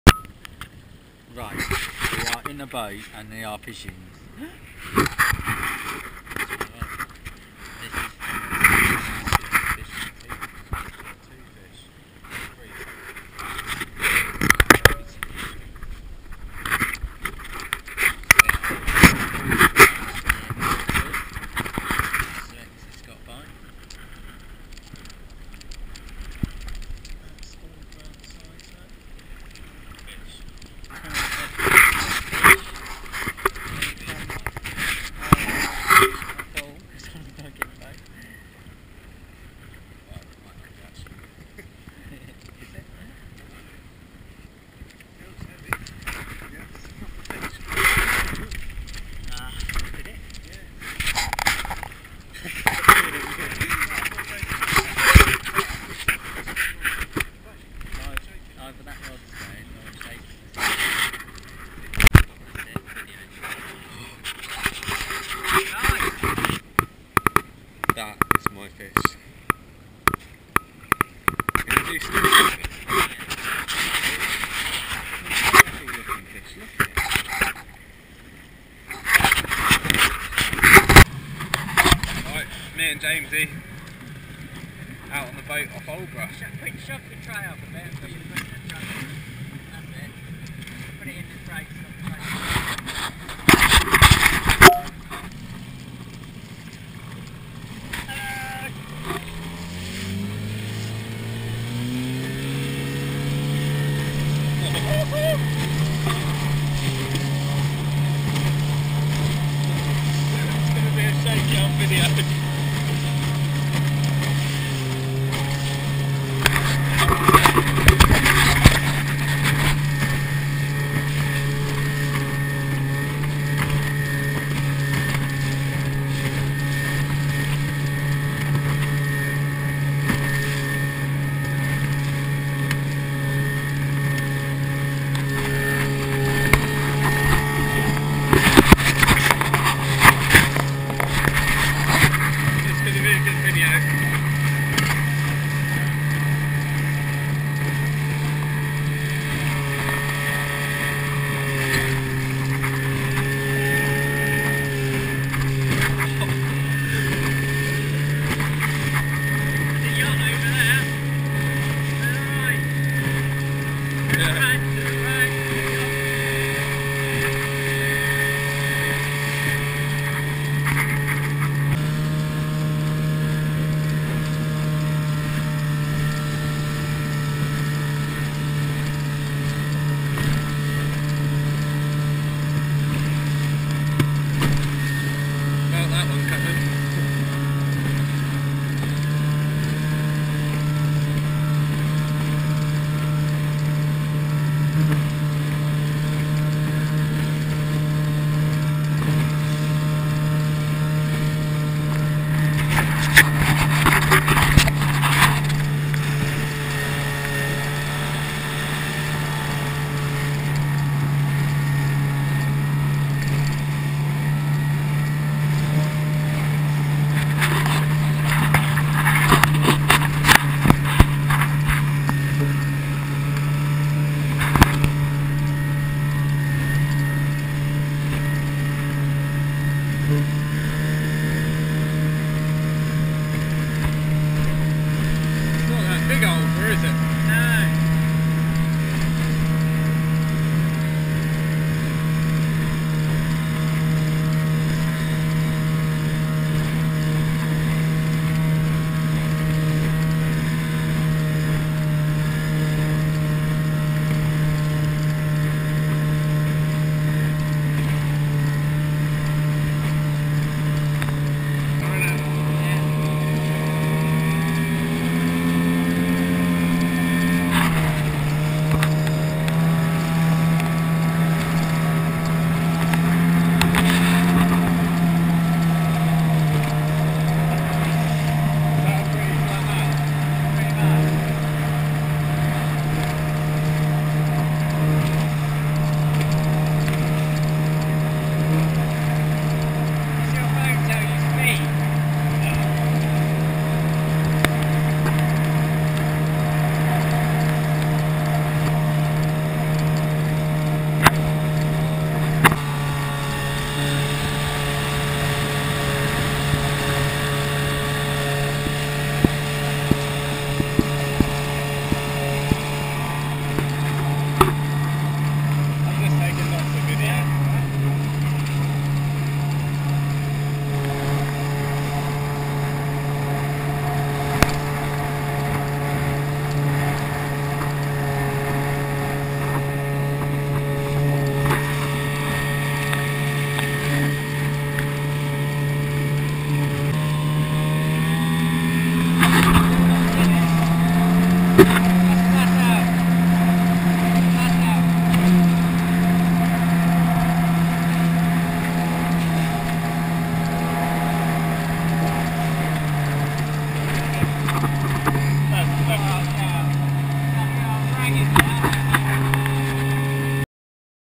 SIB fishing off of Aldeburgh 13-04-2014
A Nice sunny Day SIB fishing off of Aldeburgh with Tom, and a quick blast along the seafront. Tom caught some nice whiting and a nice Dogfish! I nearly blanked, catching only a dogfish on the last cast.